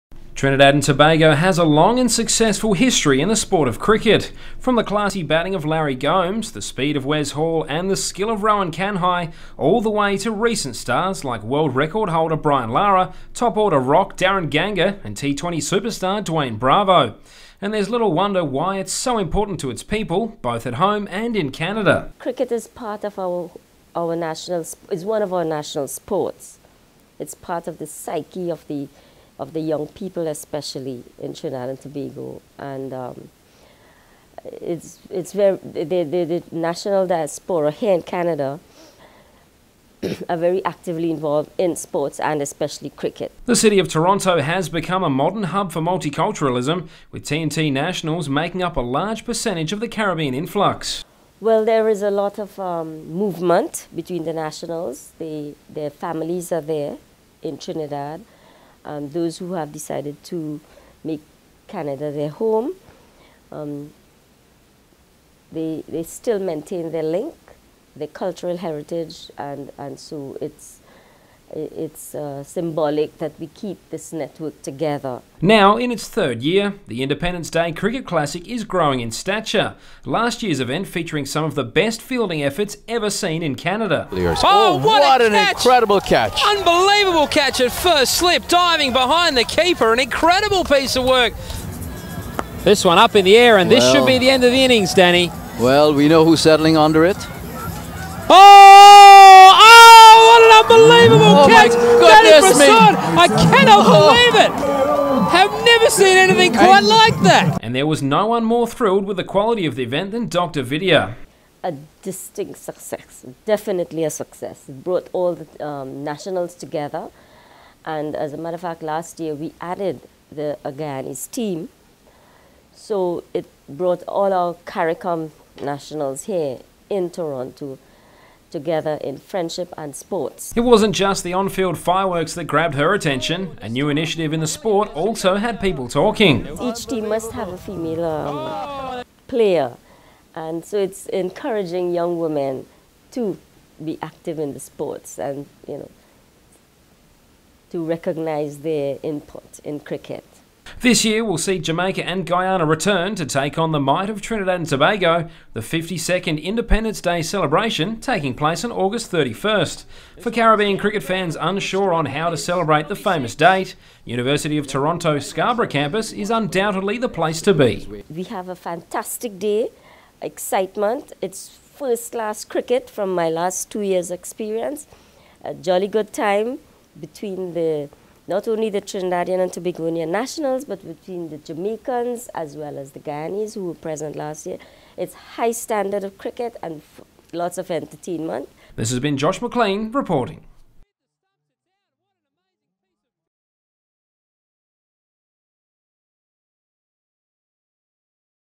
Cricket Classic 2014 - Dr. Vidhya Gyan Tota-Maharaj Interview
Consul General for the Republic of Trinidad and Tobago in Toronto